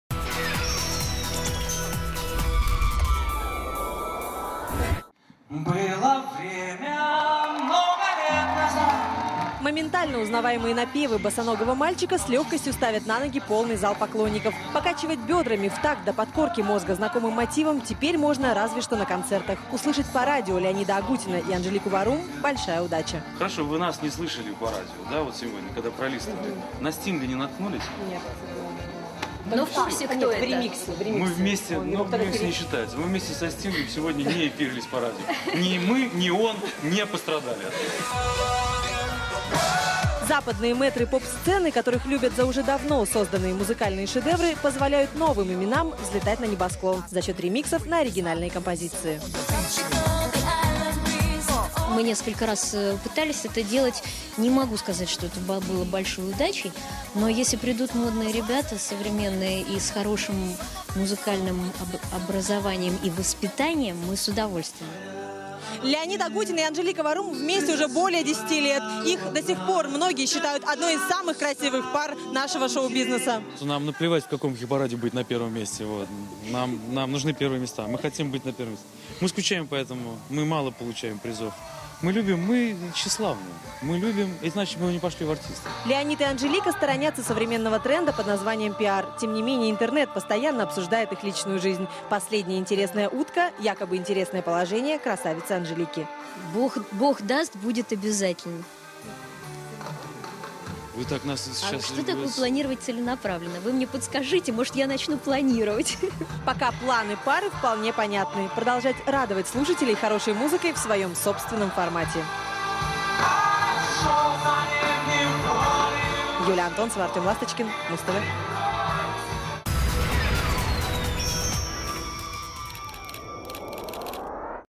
А. Варум и Л. Агутин — А будет ли мальчик?
Будет ли мальчик? Корреспондент PRO-Новостей расспрашивает Анжелику Варум и Леонида Агутина про  интересное положение